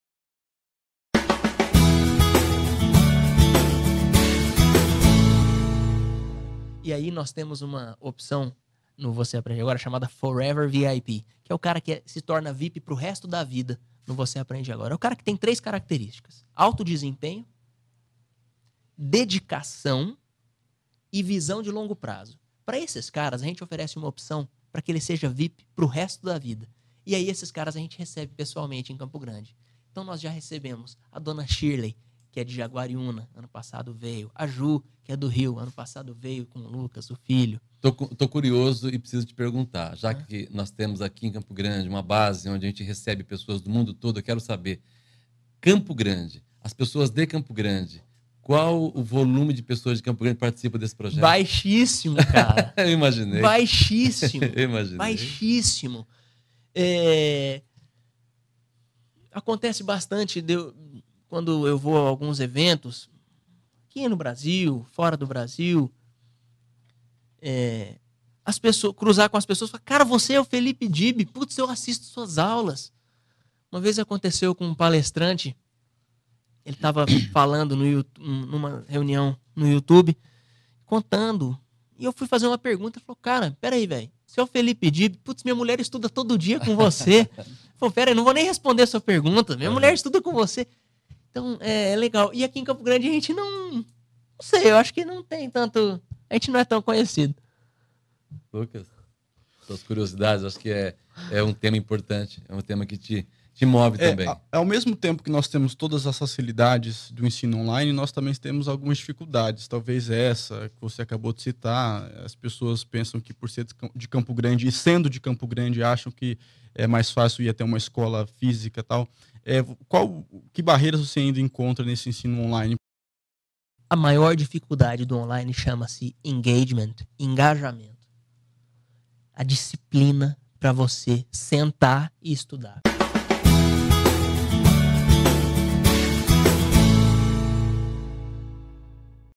Felipe Dib na CBN 6: como se tornar Forever VIP.
Aula do curso de ingles voce aprende agora
15:21 - We have an option at Você Aprende Agora…
15:23 - ...called "Forever VIP".
15:25 - The guy who becomes VIP…
15:27 - ...for the rest of his life at
15:29 - It's the guy who has 3 characteristics:
15:31 - High performance.
15:33 - Dedication and long-term vision.
15:36 - To these guys we offer an option…
15:38 - ...to be VIP for the rest of his/her life.
15:40 - And these people we receive face-to-face…
15:42 - ...in Campo Grande.
15:44 - We've already received Shirley…
15:46 - ...from Jaguariúna, came last year.
15:48 - Ju from Rio came last year…
15:50 - ...with Lucas her son.
15:52 - I'm curious and I need to ask you…
15:54 - ...once we have here in Campo Grande…
15:56 - ...a base where we receive people from all over the world.
15:58 - I want to know.
16:00 - The people from Campo Grande.
16:02 - What's the % of people from Campo Grande participate?
16:04 - Super low!
16:06 - I imagined!
16:08 - Super low!
16:13 - It happens a lot…
16:15 - ...when I go to some events…
16:17 - ...here in Brazil, overseas…
16:23 - ...I come across people and they say:
16:25 - "You're Felipe Dib! I watch your classes!"
16:28 - Once it happened with a speaker.
16:30 - He was speaking…
16:32 - ...at a meeting on YouTube headquarters.
16:34 - I asked a question.
16:37 - He said: wait, you're Felipe Dib?
16:39 - My wife studies every day with you!
16:43 - My wife studies with you!
16:45 - It's cool!
16:47 - And here in Campo Grande…
16:49 - ...we're not much known.
16:53 - Lucas, your curiosities…
16:55 - ...I think it's an important topic.
16:57 - At the same time…
16:59 - ...we have all the facilities…
17:01 - ...of e-learning…
17:03 - ...we also have difficulties.
17:05 - Maybe this one you've just mentioned.
17:07 - People think that being from Campo Grande…
17:10 - ...it's easier to go to a physical location.
17:13 - Which barriers do you find…
17:15 - ...on e-learning?